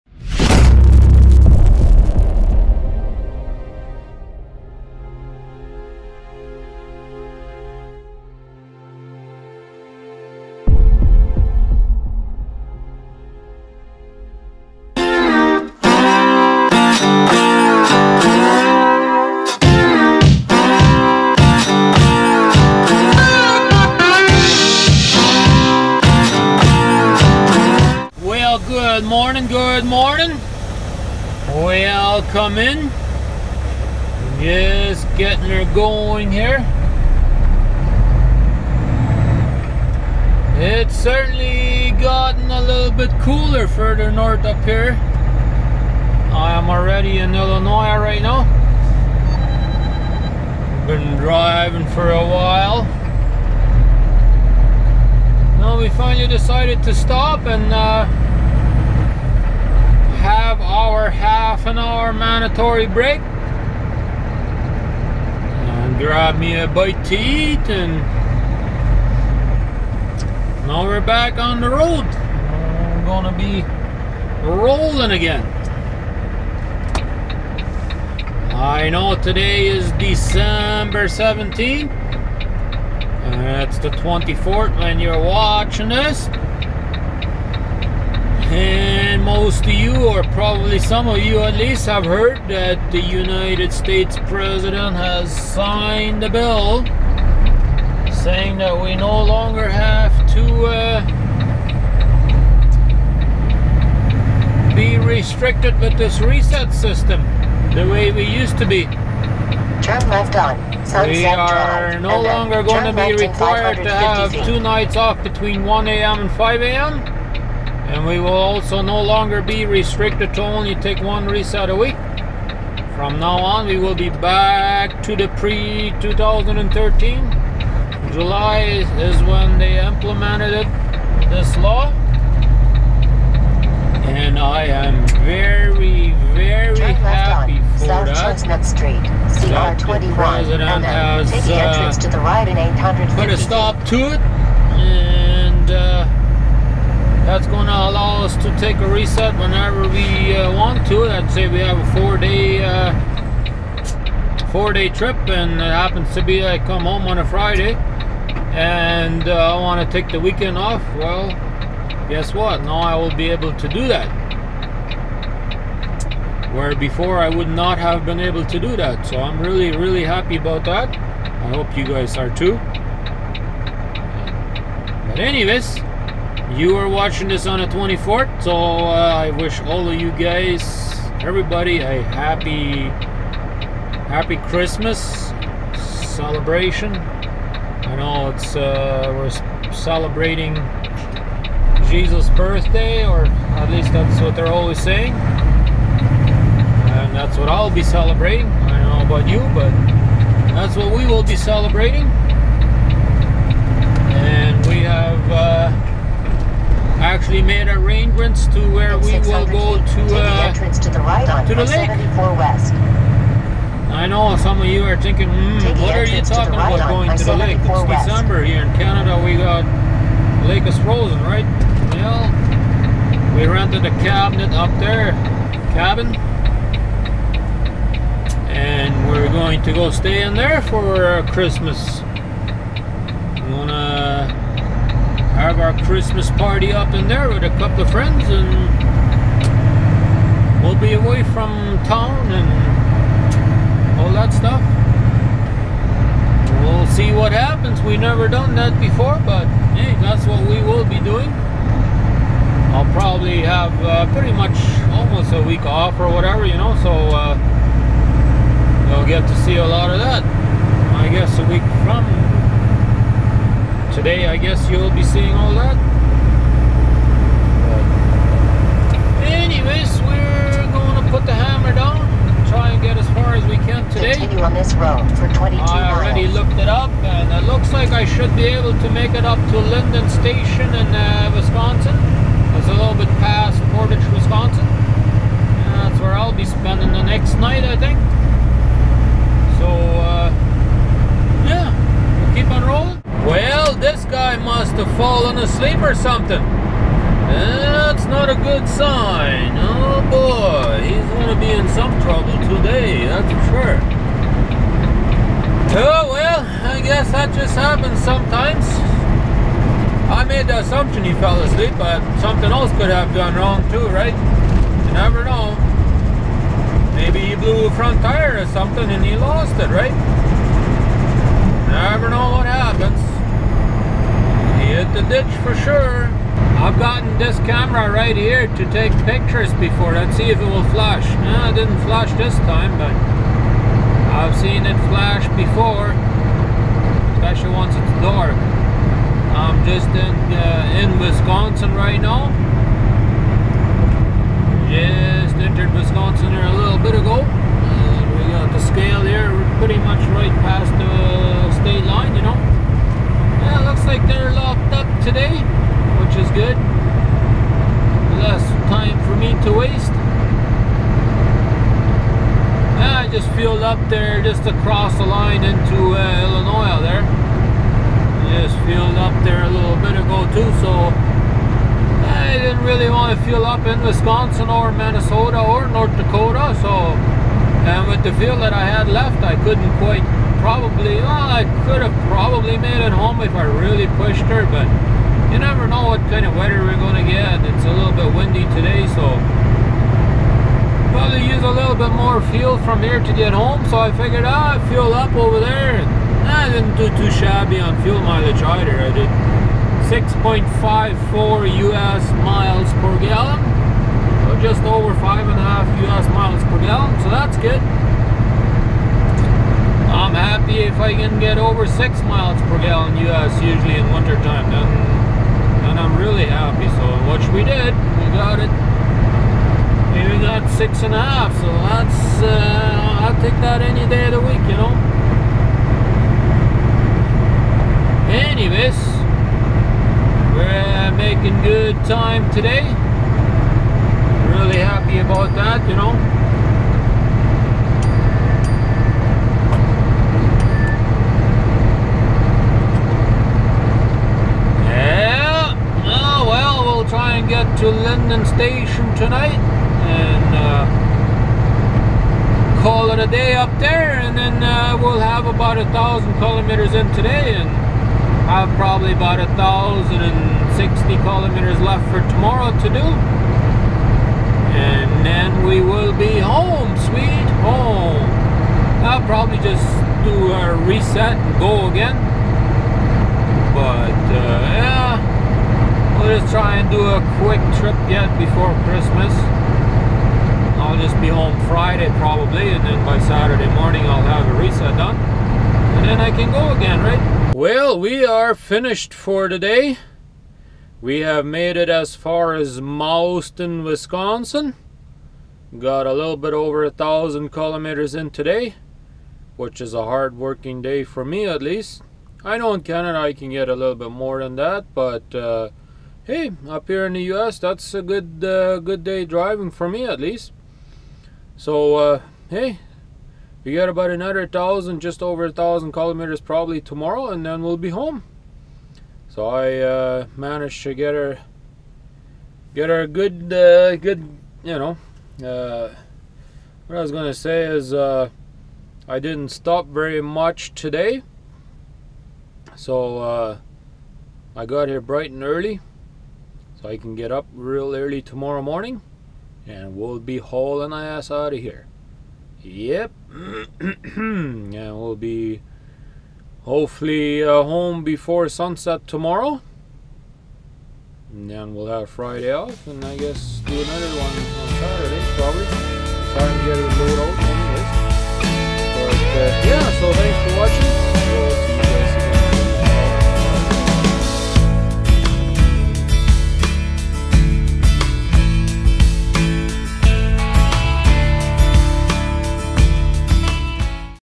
Trucker Rudi 12.17.14 president Obama has signed the bill
president Obama has signed the bill so we no longer are restricted to the restart system